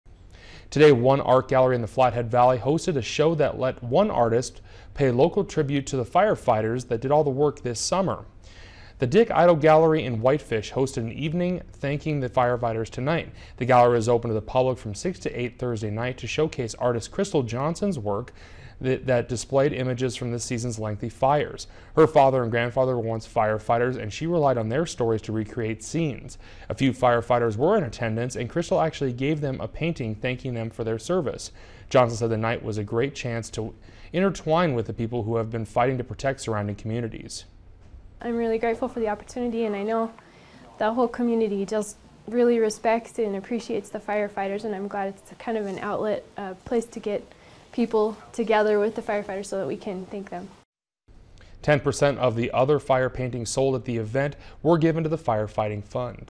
Flathead Valley artist thanks firefighters through art
Artist Crystal Johnson said the night was a great chance to intertwine with the people who have been fighting to protect the surrounding communities.